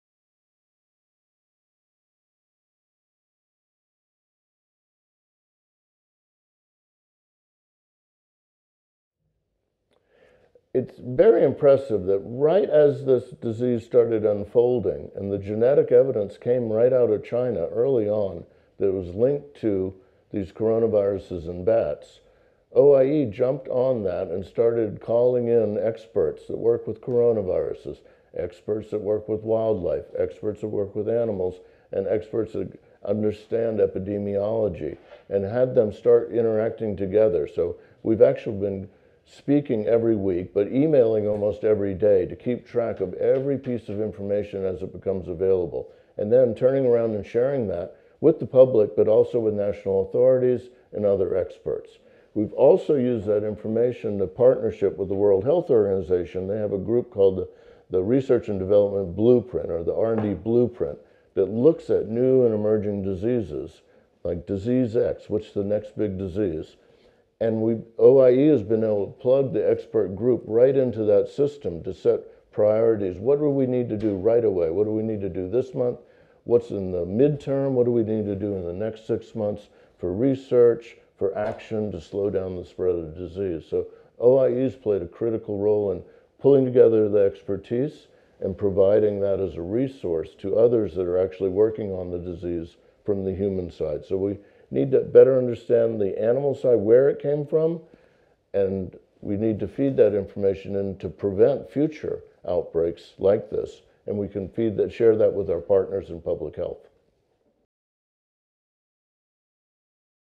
¿Qué ha estado haciendo la OIE en respuesta a COVID19?
A la luz de la importancia de UnaSolaSalud, la OIE encabeza el trabajo con expertos pertinentes cada vez que emergen enfermedades en los animales.